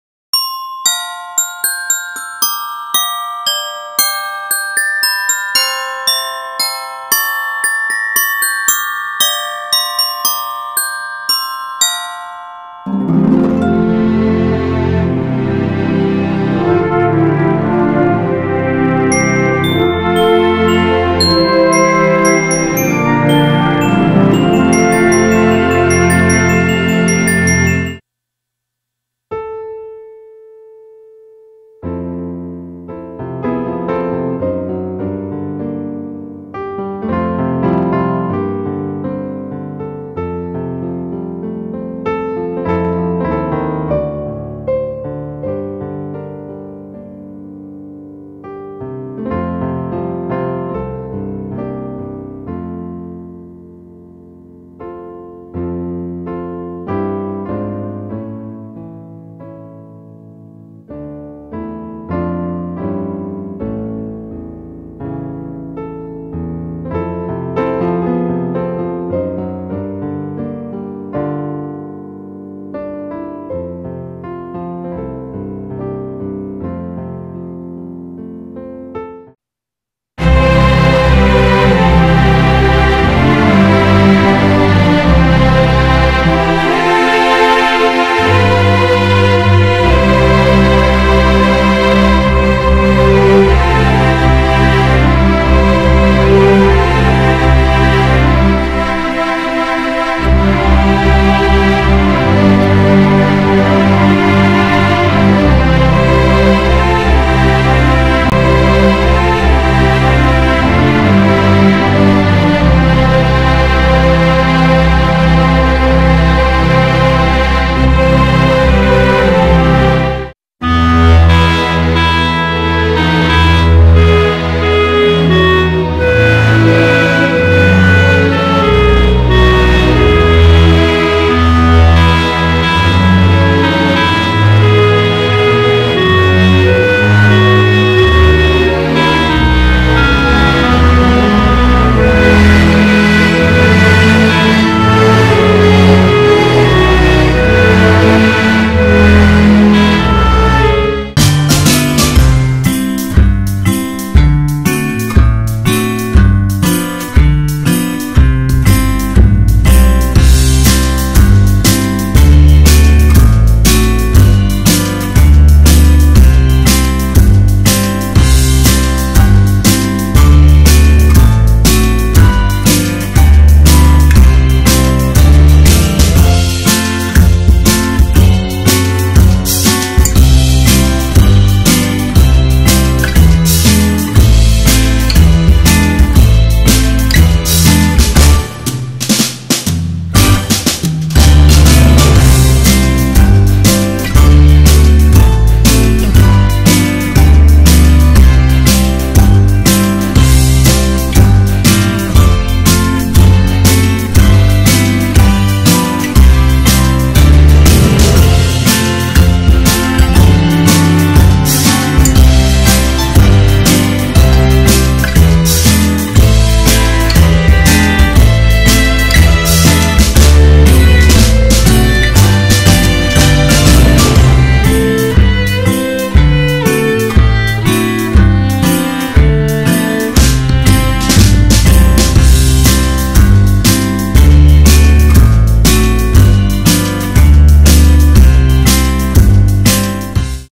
Havanese Nativity
Havanese dogs nativity play

A fun play for Christmas with a Havanese twist and some very cute puppies plus ‘out takes.’